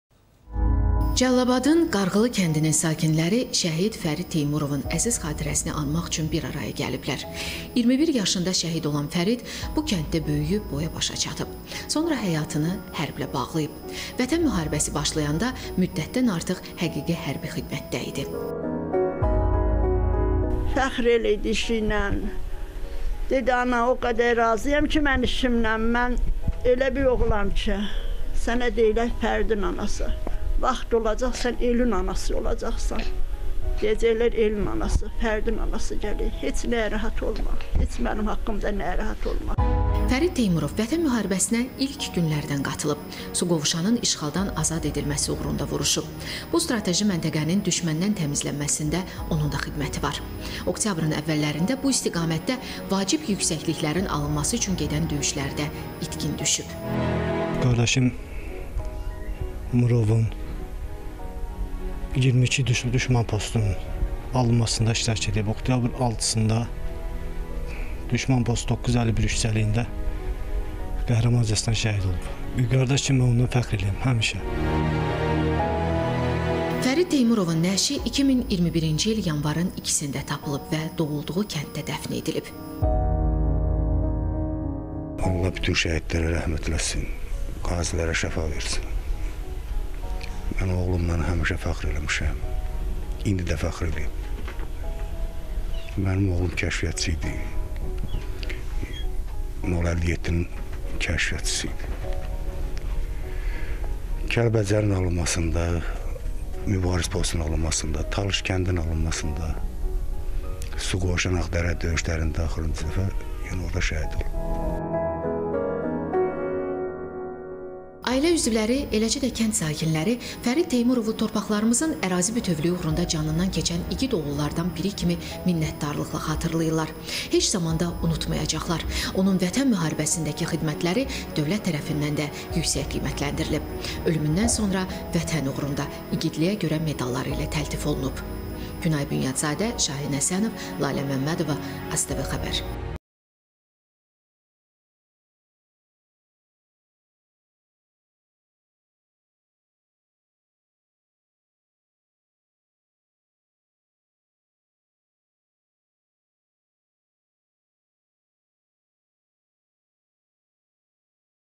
21 yaşında şəhidlik zirvəsinə ucalan Fərid Teymurov
Azərbaycan Ordusunun Vətən müharibəsində qazandığı zəfər tariximizin ən parlaq səhifələrindən biridir. 44 gün davam edən savaş torpaqlarımızın işğaldan azad olunması ilə nəticələndi, xalqımızın 30 illik həsrətinə son qoydu. Bu yolda canından keçən şəhidlərin də xatirəsi hər zaman xatırlanacaq. Belə qəhrəmanlardan biri - Fərid Teymurovun anım günü keçirilib.